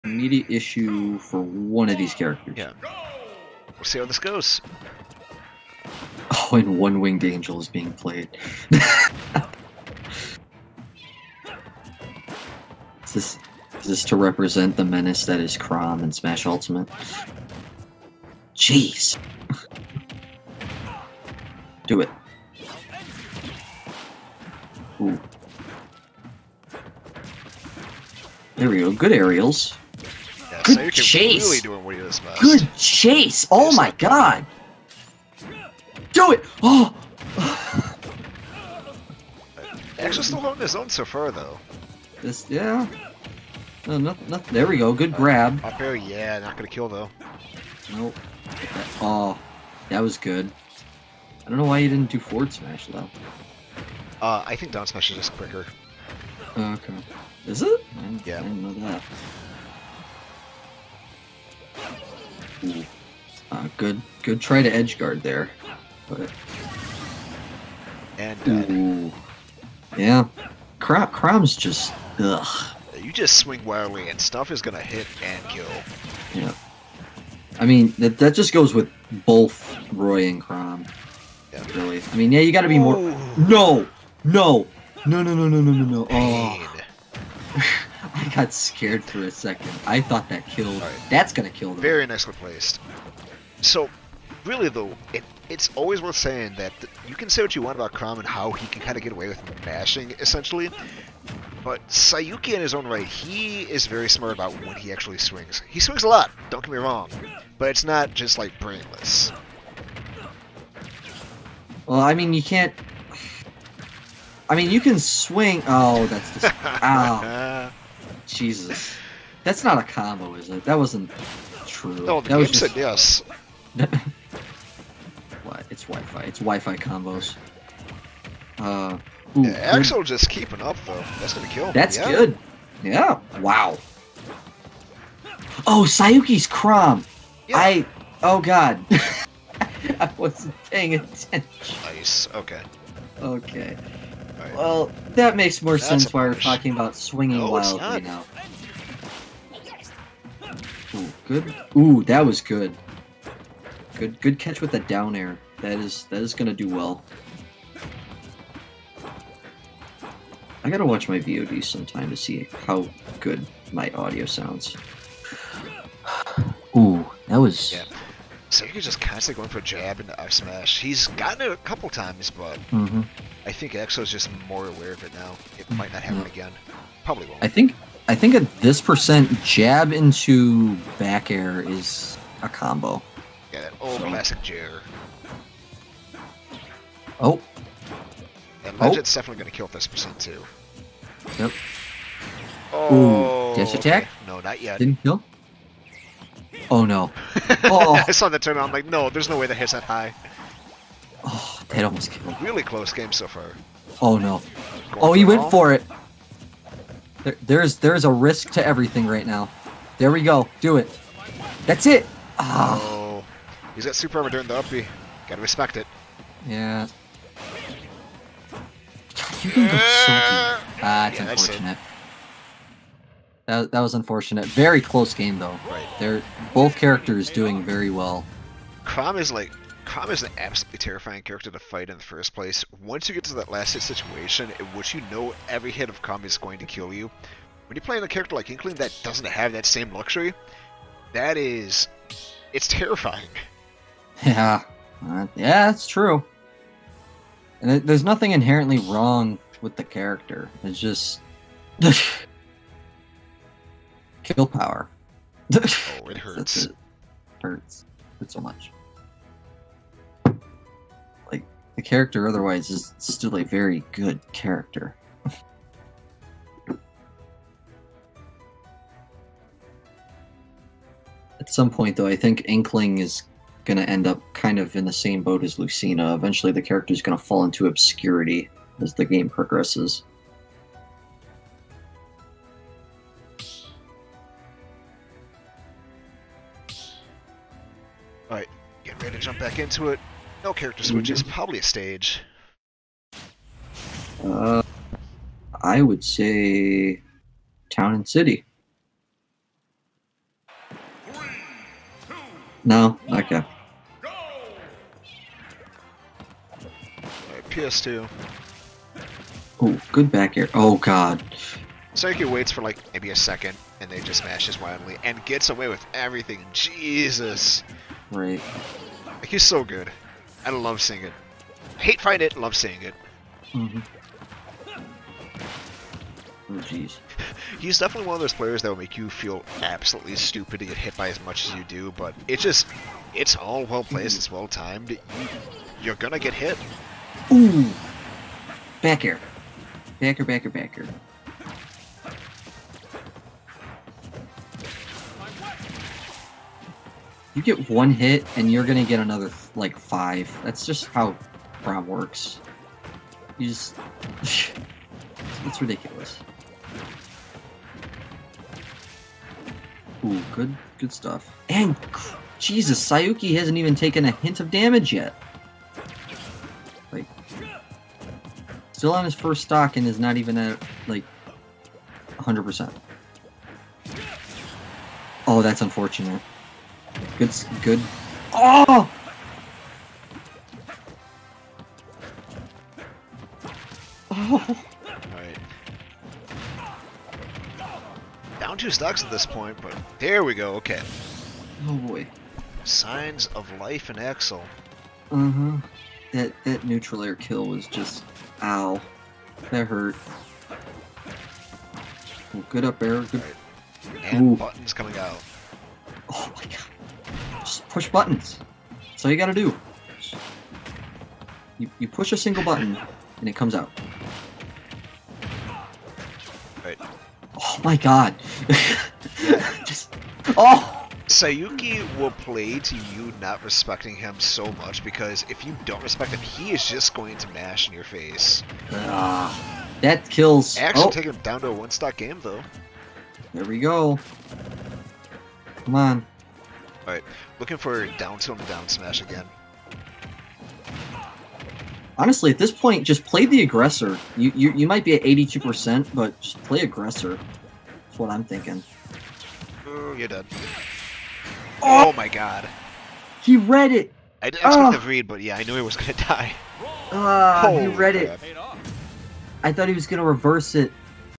Friday Night Netplay #34 - Ladder - Sayuki(Chrom) Vs. Axel(Inkling)
Friday Night Netplay #34 | 1/01/21
Romeoville, Illinois
Sayuki: @Sayuki145
Axel: @energy_shadow